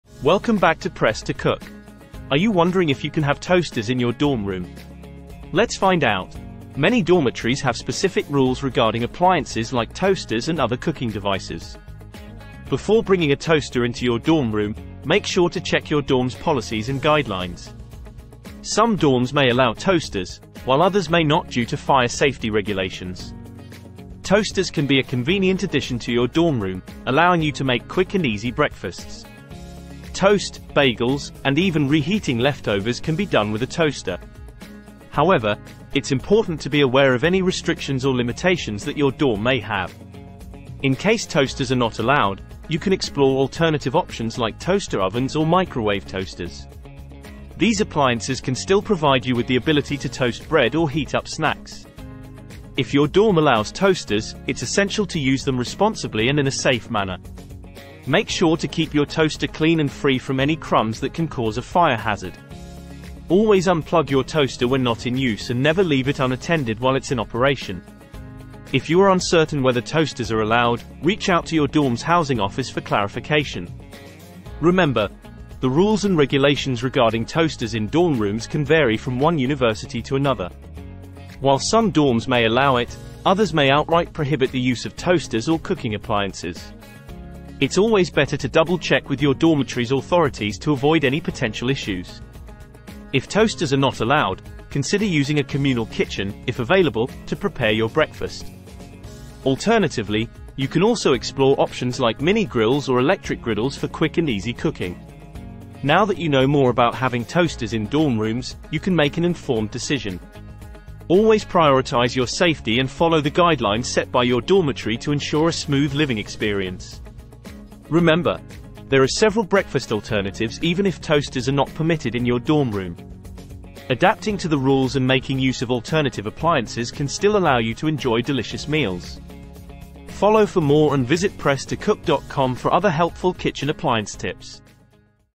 The Truth About Toasters in Dorm Rooms
Do you love toast but live in a dorm? Find out if you can have a toaster in your room and what to do if it’s not allowed. the best website for kitchen appliances.

I hope this helps. If you need more assistance, please let me know. 😊